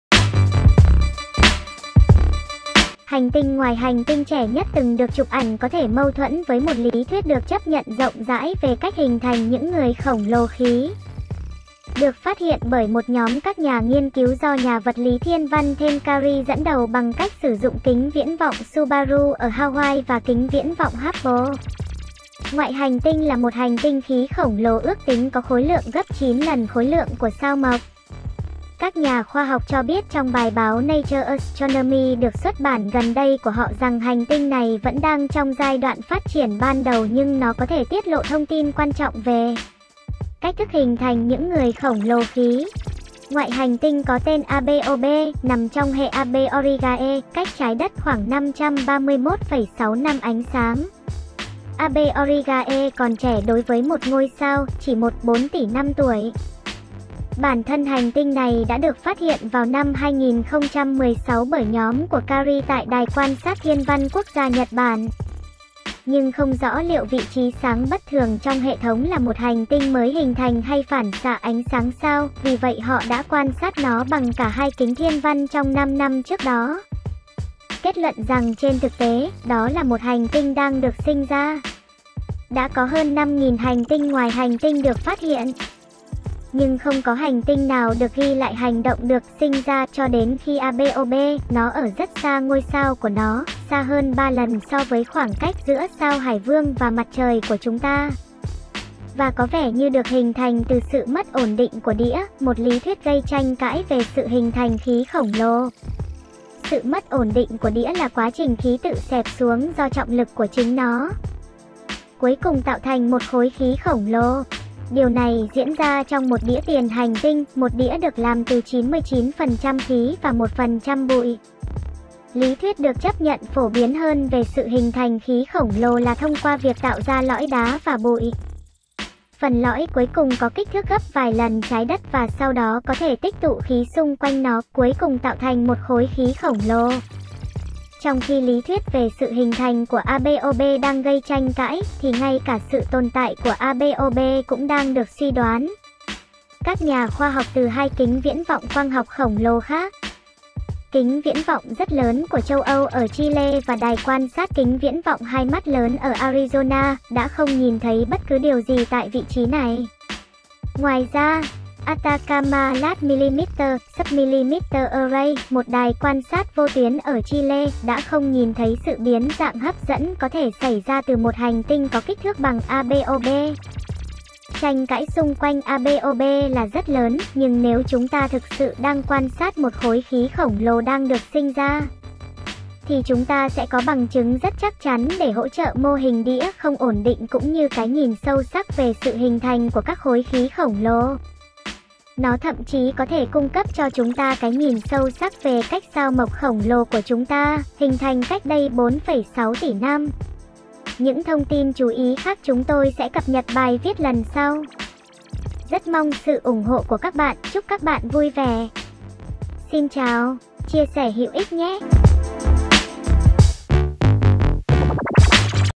Hành tinh khí khổng lồ tiềm năng được phát hiện trong quá trình hình thành
Hành tinh ngoài hành tinh trẻ nhất từng được chụp ảnh có thể mâu thuẫn với một lý thuyết được chấp nhận rộng rãi về cách hình thành những người khổng lồ khí.

Được phát hiện bởi một nhóm các nhà nghiên cứu do nhà vật lý thiên văn Thayne Currie dẫn đầu bằng cách sử dụng Kính viễn vọng Subaru ở Hawaii và Kính viễn vọng Hubble, ngoại hành tinh là một hành tinh khí khổng lồ ước tính có khối lượng gấp 9 lần khối lượng của Sao Mộc. Các nhà khoa học cho biết trong bài báo Nature Astronomy được xuất bản gần đây của họ rằng hành tinh này vẫn đang trong giai đoạn phát triển ban đầu nhưng nó có thể tiết lộ thông tin quan trọng về cách thức hình thành những người khổng lồ khí.

Ngoại hành tinh có tên AB Aur B, nằm trong hệ AB Aurigae, cách Trái đất khoảng 531,6 năm ánh sáng. AB Aurigae còn trẻ đối với một ngôi sao, chỉ 1-4 tỷ năm tuổi. Bản thân hành tinh này đã được phát hiện vào năm 2016 bởi nhóm của Currie tại Đài quan sát Thiên văn Quốc gia Nhật Bản, nhưng không rõ liệu vị trí sáng bất thường trong hệ thống là một hành tinh mới hình thành hay phản xạ ánh sáng sao, vì vậy họ đã quan sát nó bằng cả hai kính thiên văn trong 5 năm trước đó. kết luận rằng trên thực tế, đó là một hành tinh đang được sinh ra.

Đã có hơn 5.000 hành tinh ngoài hành tinh được phát hiện, nhưng không có hành tinh nào được ghi lại hành động được “sinh ra” cho đến khi AB Aur B. Nó ở rất xa ngôi sao của nó, xa hơn ba lần so với khoảng cách giữa Sao Hải Vương và Mặt trời của chúng ta, và có vẻ như được hình thành từ sự mất ổn định của đĩa, một lý thuyết gây tranh cãi về sự hình thành khí khổng lồ.

Sự mất ổn định của đĩa là quá trình khí tự xẹp xuống do trọng lực của chính nó, cuối cùng tạo thành một khối khí khổng lồ. Điều này diễn ra trong một đĩa tiền hành tinh, một đĩa được làm từ 99% khí và 1% bụi. Lý thuyết được chấp nhận phổ biến hơn về sự hình thành khí khổng lồ là thông qua việc tạo ra lõi đá và bụi. Phần lõi cuối cùng có kích thước gấp vài lần Trái đất và sau đó có thể tích tụ khí xung quanh nó, cuối cùng tạo thành một khối khí khổng lồ.

Trong khi lý thuyết về sự hình thành của AB Aur B đang gây tranh cãi, thì ngay cả sự tồn tại của AB Aur B cũng đang được suy đoán. Các nhà khoa học từ hai kính viễn vọng quang học khổng lồ khác, Kính viễn vọng Rất lớn của Châu Âu ở Chile và Đài quan sát Kính viễn vọng Hai mắt Lớn ở Arizona, đã không nhìn thấy bất cứ điều gì tại vị trí này . Ngoài ra, Atacama Large Millimeter / submillimeter Array, một đài quan sát vô tuyến ở Chile, đã không nhìn thấy sự biến dạng hấp dẫn có thể xảy ra từ một hành tinh có kích thước bằng AB Aur B.

Tranh cãi xung quanh AB Aur B là rất lớn, nhưng nếu chúng ta thực sự đang quan sát một khối khí khổng lồ đang được sinh ra, thì chúng ta sẽ có bằng chứng rất chắc chắn để hỗ trợ mô hình đĩa không ổn định cũng như cái nhìn sâu sắc về sự hình thành của các khối khí khổng lồ. Nó thậm chí có thể cung cấp cho chúng ta cái nhìn sâu sắc về cách sao Mộc khổng lồ của chúng ta, hình thành cách đây 4,6 tỷ năm.